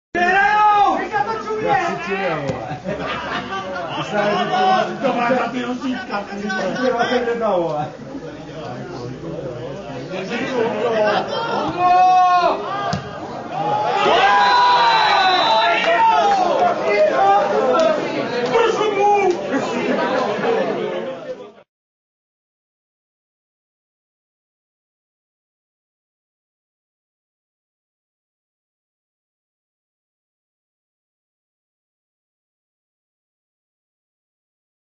Šluknovský fotbal 2008/9 : Šluknov - Modlany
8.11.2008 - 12.kolo I.A třídy - poslední branka zápasu, kterou hosté upravují na 2:4